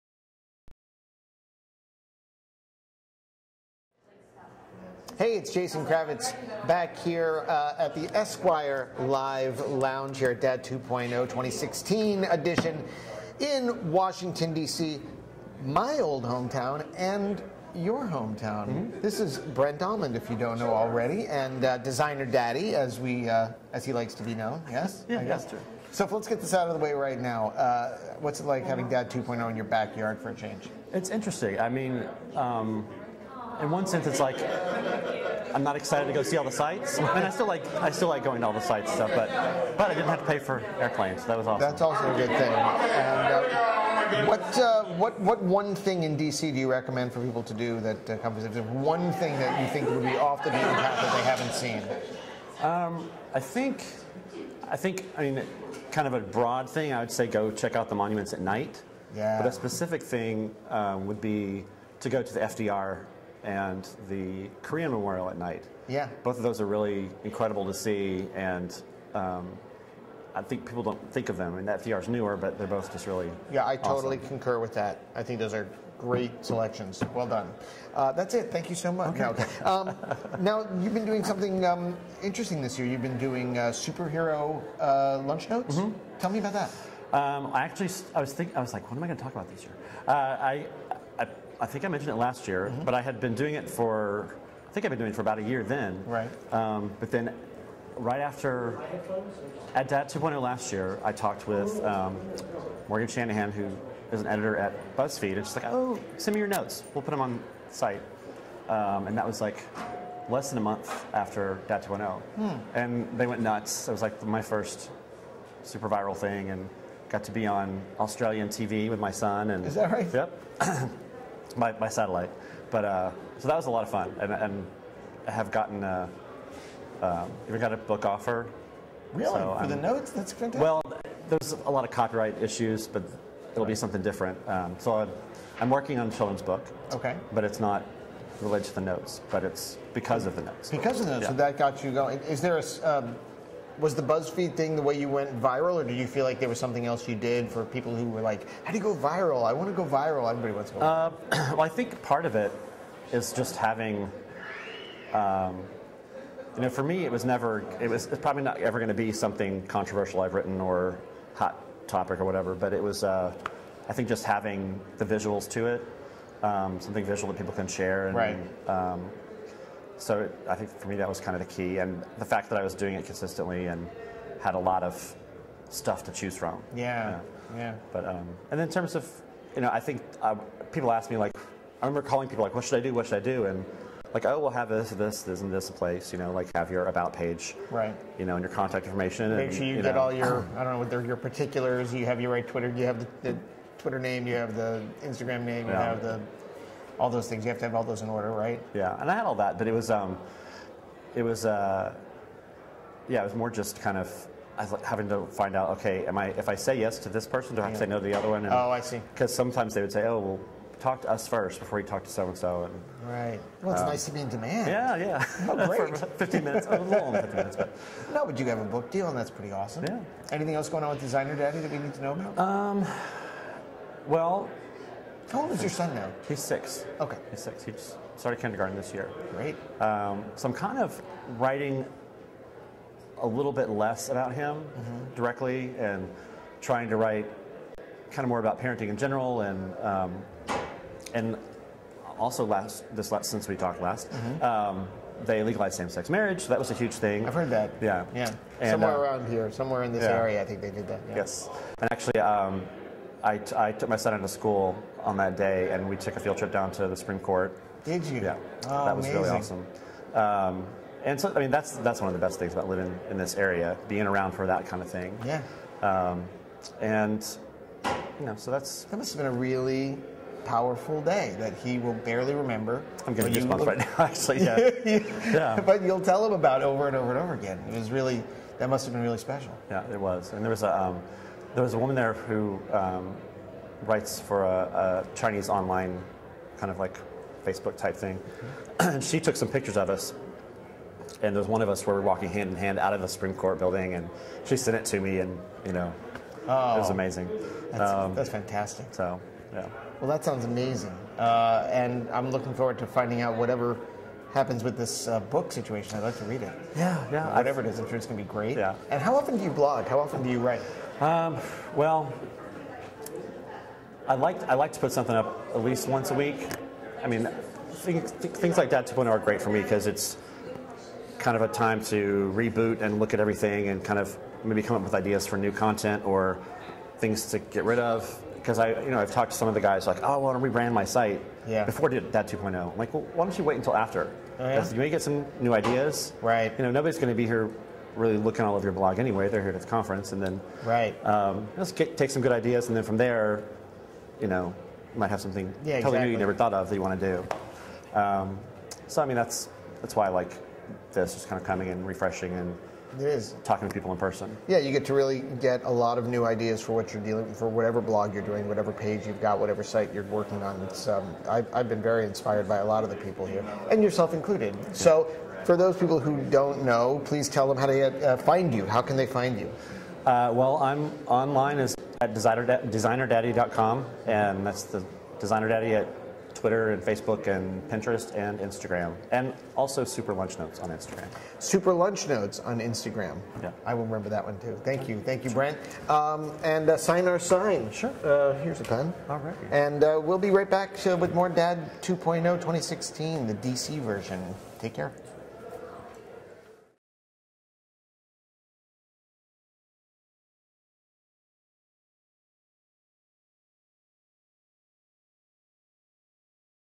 Brent Almond and Jason Kravits at Dad2Summit Esquire Live Lounge
Brent Almond and Jason Kravits at Dad2Summit Esquire Live Lounge - Captured Live on Ustream